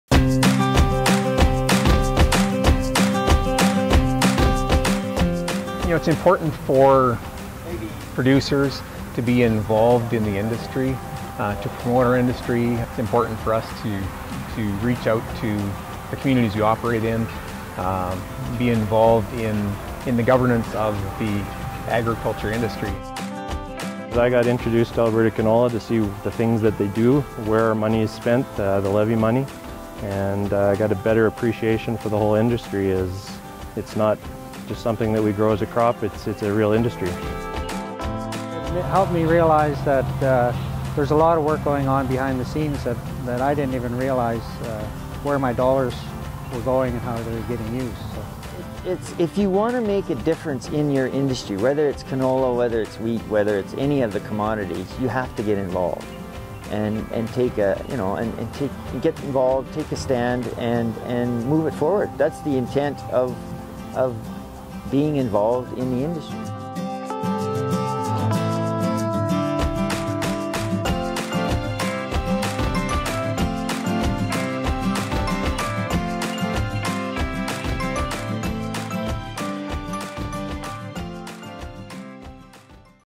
Alberta Canola - Let Your Name Stand: Becoming a director
Alberta Canola is seeking four canola growers to serve on the board of directors.